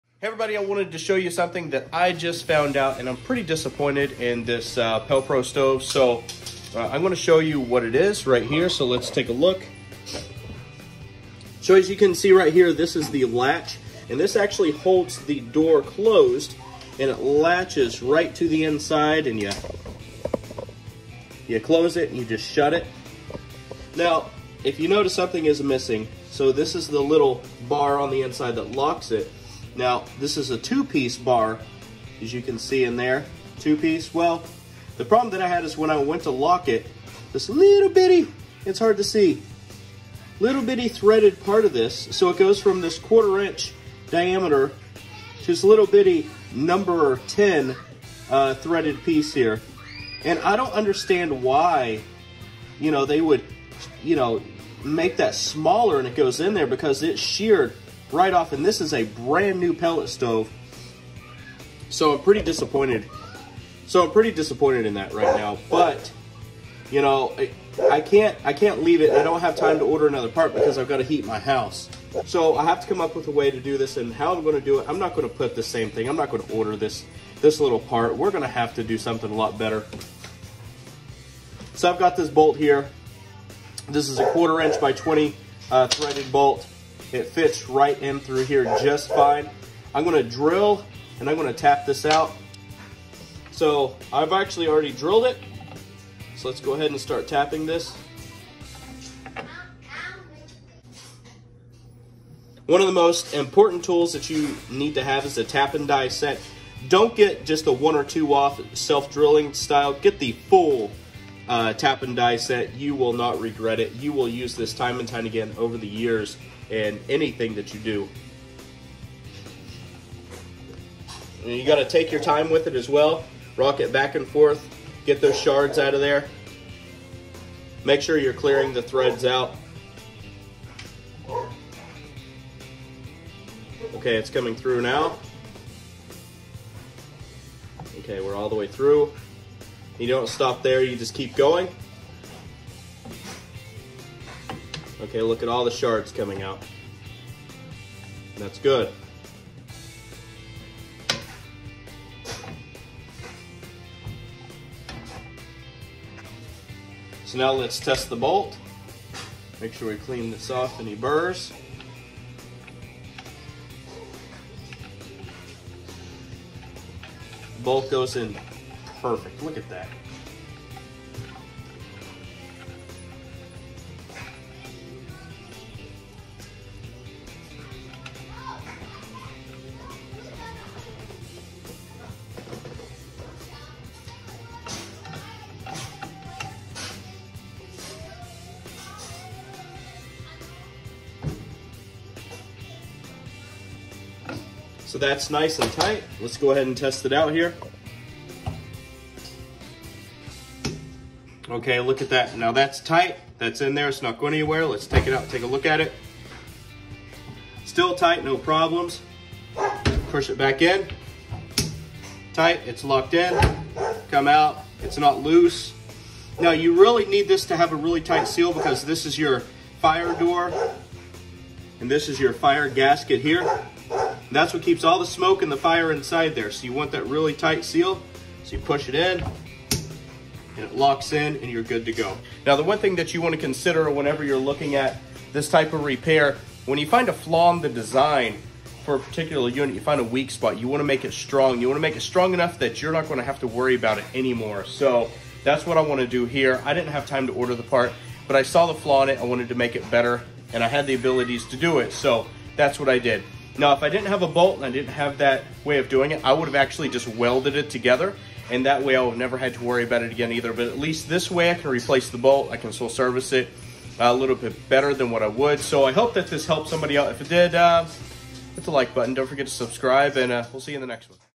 Pelpro Pellet stove door fix
The fire door latch shared off, now let’s fix it and make it better than it was before. The pelpro pellet is a great stove now let’s improve it.